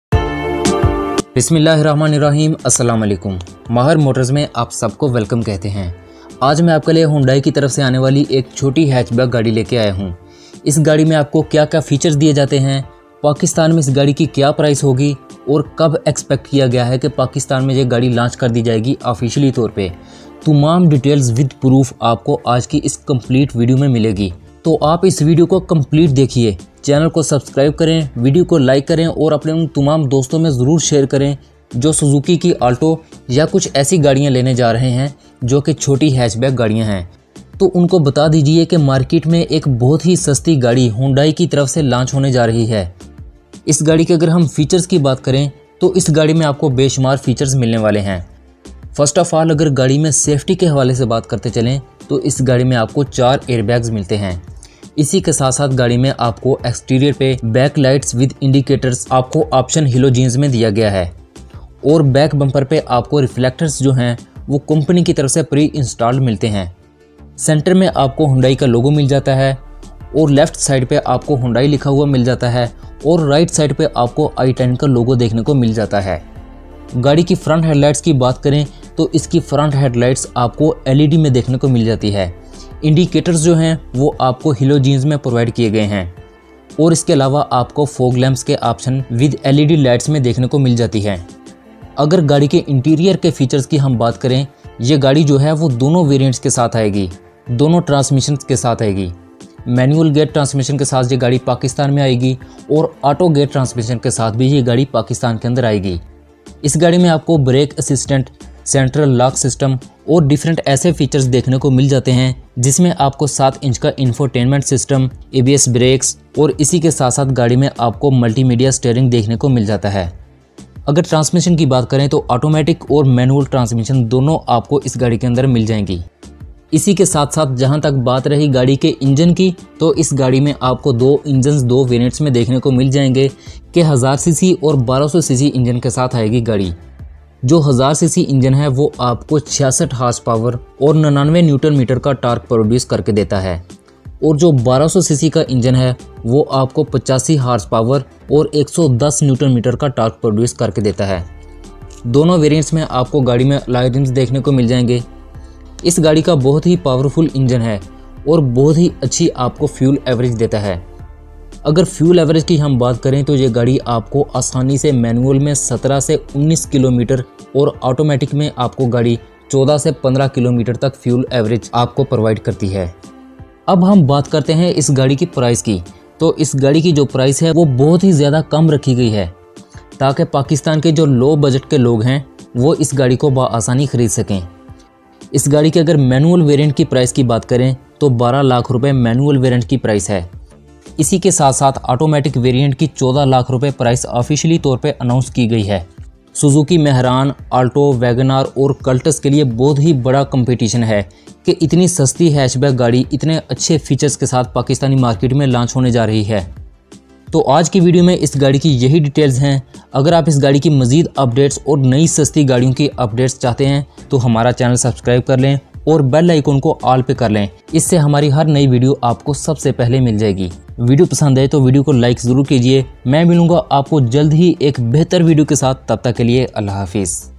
Hyundai i10 hatchback Coming To Pakistan With Very Cheap Price and Features
Here is the video of hyundai hatchback car with very cheap price and features in pakistan.